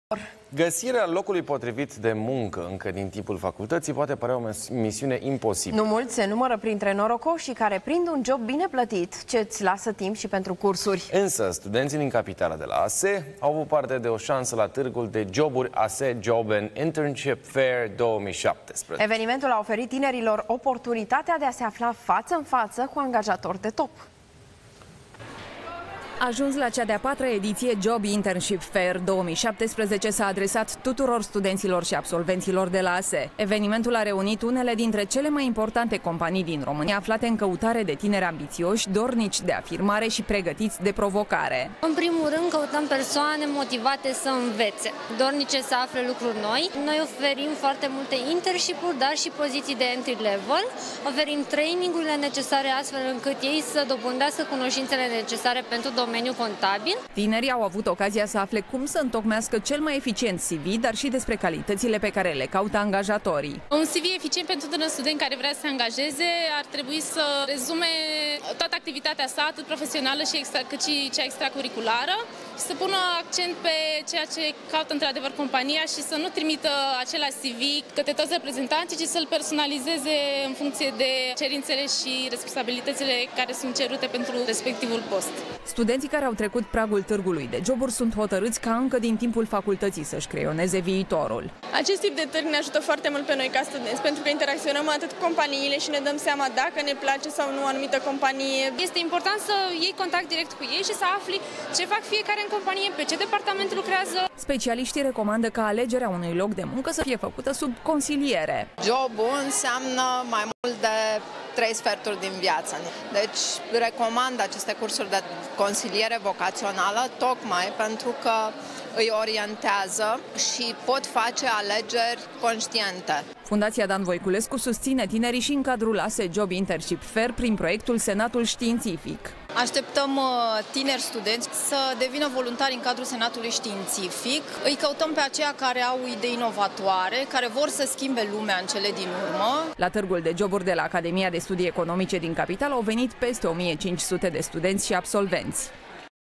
Antena 1: Găsirea locului de muncă dorit încă din timpul facultăţii poate părea o misiune imposibilă
Găsirea locului de muncă dorit încă din timpul facultăţii poate părea o misiune imposibilă. Nu mulți se numără printre norocoșii care prind un job bine plătit ce îți lasă timp și pentru cursuri.